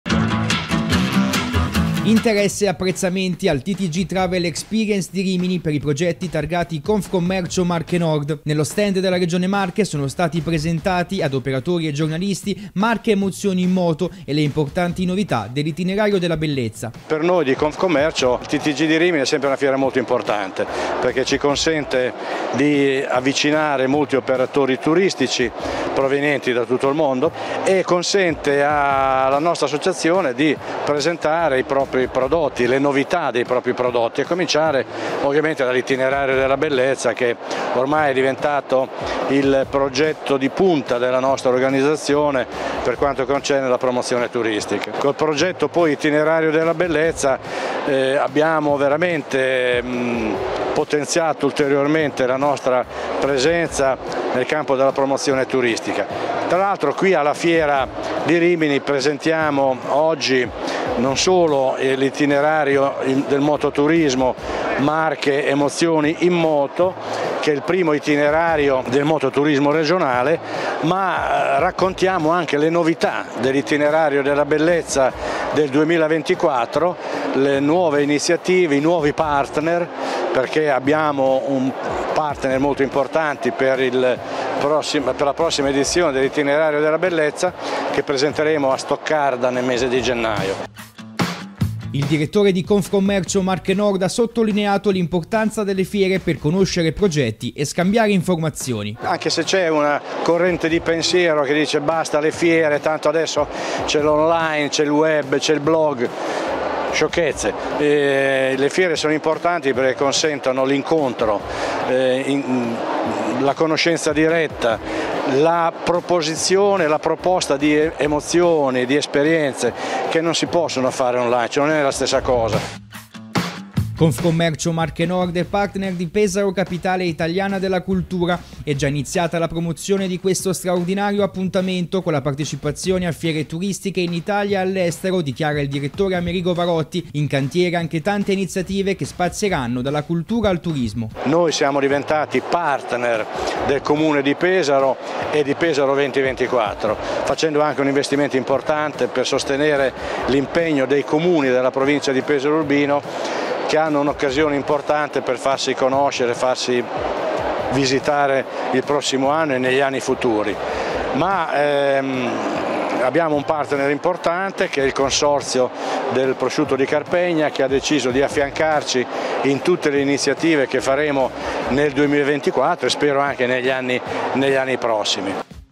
Confcommercio Marche Nord alla Fiera TTG di Rimini
A Rimini, nello stand della Regione Marche, Confcommercio Marche Nord ha presentato due progetti: ‘Marche, emozioni in moto’ e l'Itinerario della Bellezza'.